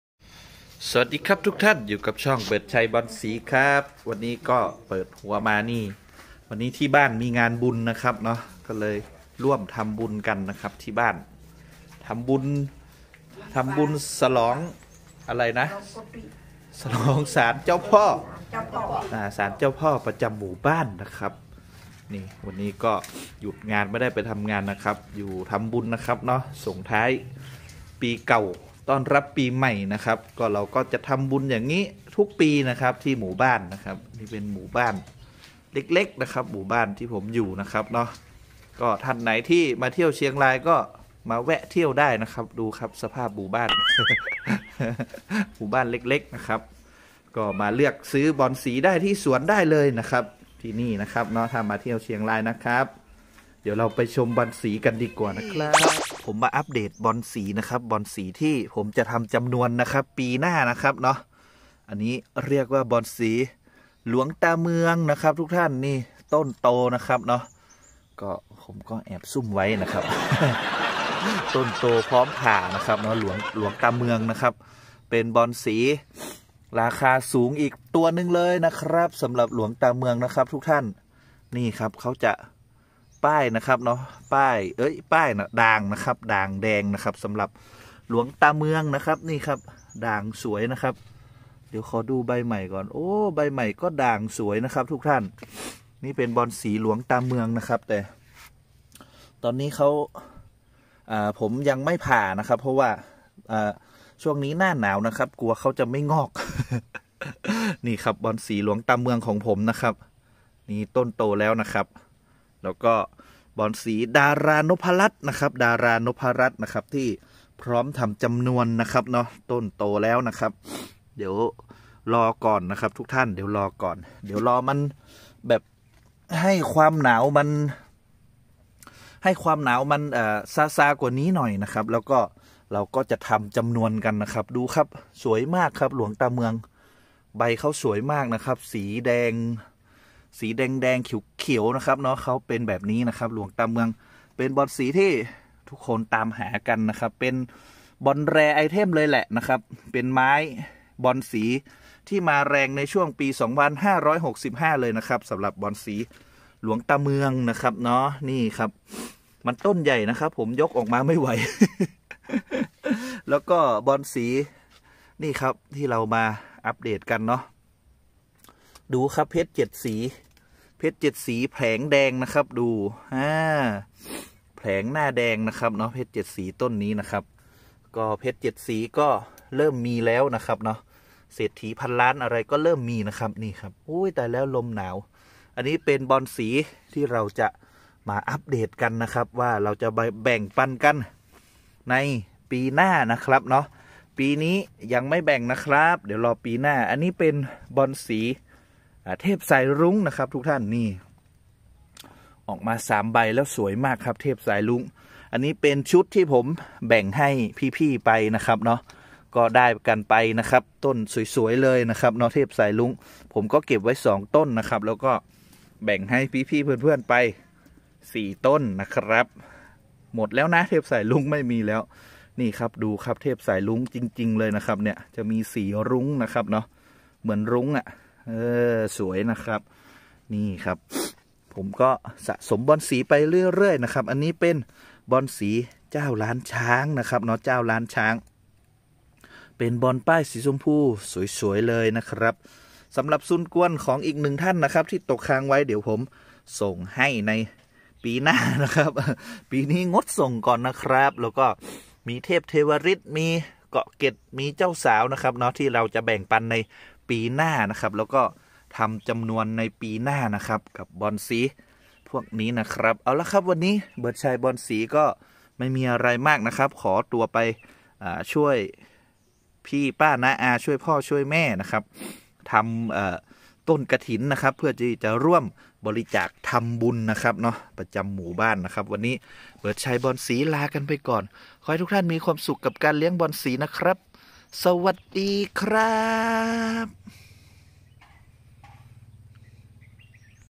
มาอัพเดตบอนสีที่เราจะทำจำนวนในปีหน้ากันครับ|เบิดชัย บอนสี
วันนี้เรามาอัพเดตบอนสีที่เราจะมาทำจำนวนในปีหน้ากันครับมาดูกันครับว่าจะมีต้นบอนสีอะไรบ้างครับ